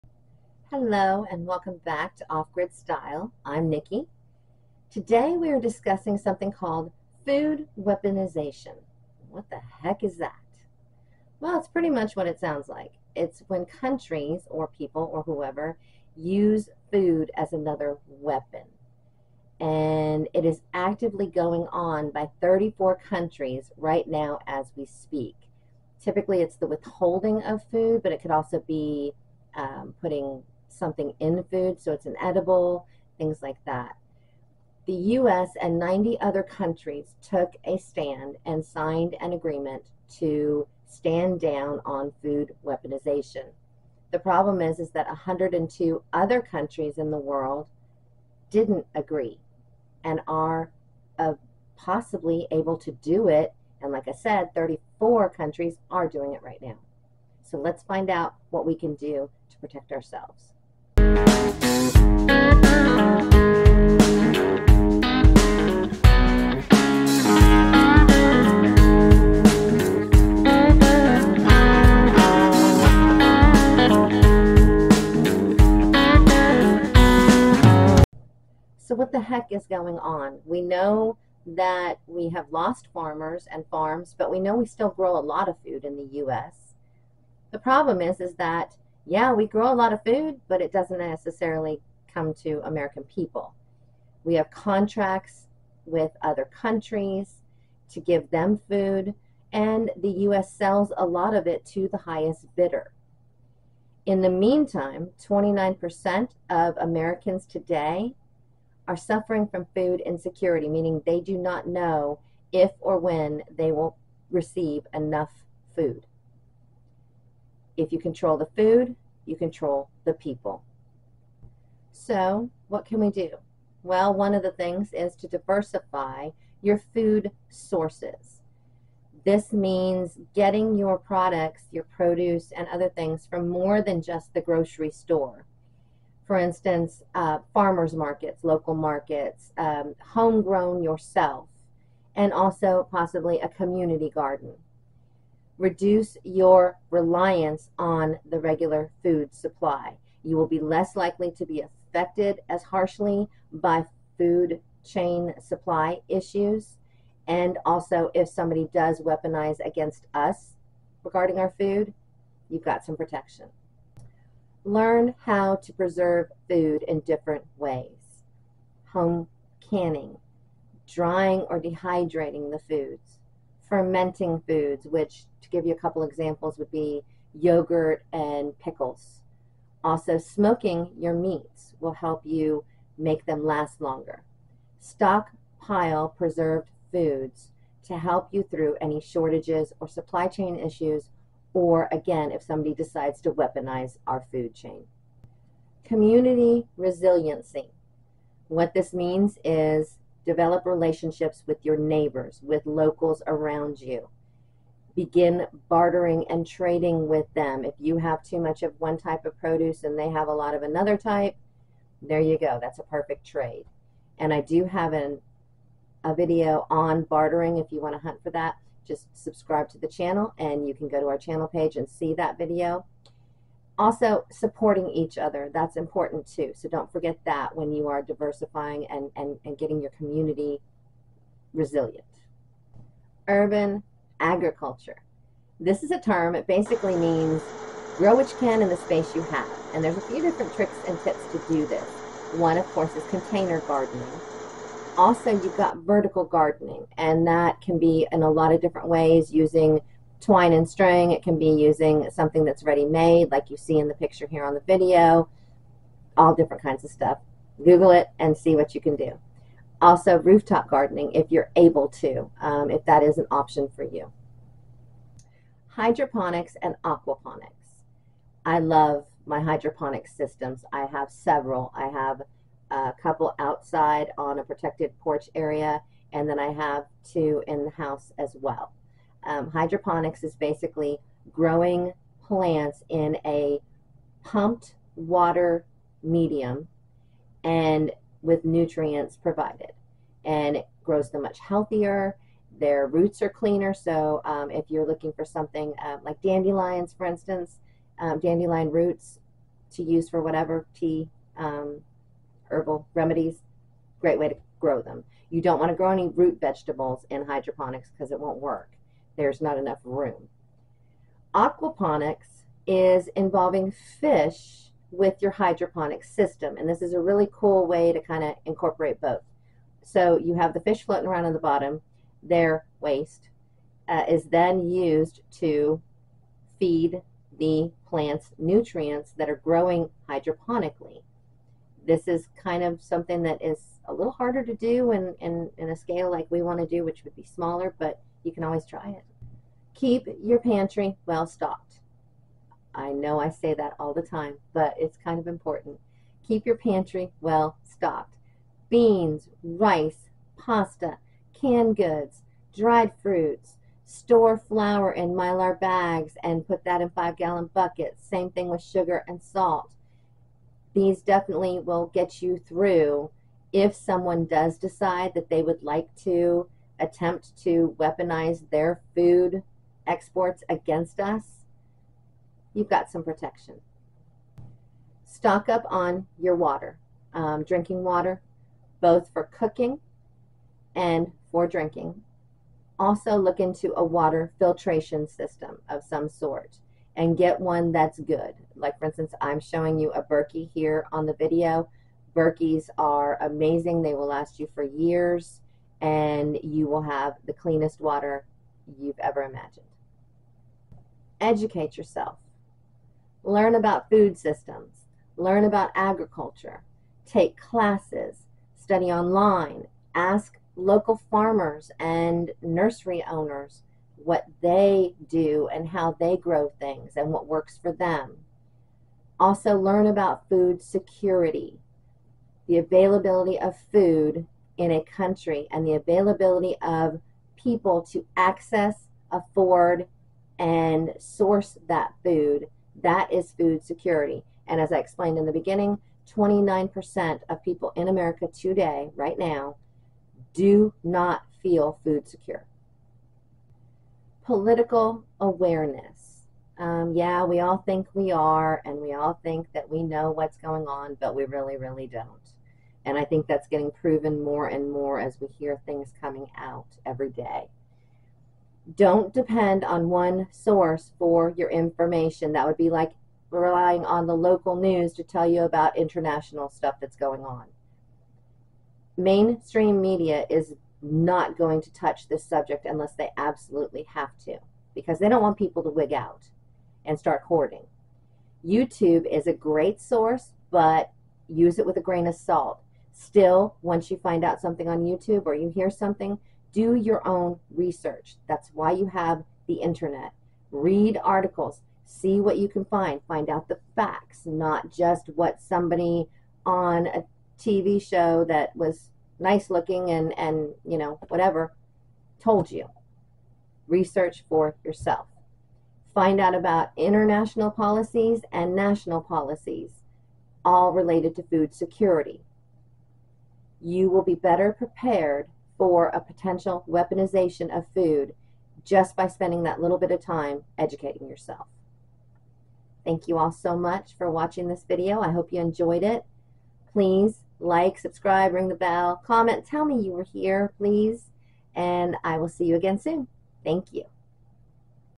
YOUR FOOD IS NOW BEING USED AGAINST YOU.
YOUR FOOD IS NOW BEING USED AGAINST YOU. Countries are weaponizing food every day, and no one is talking about it.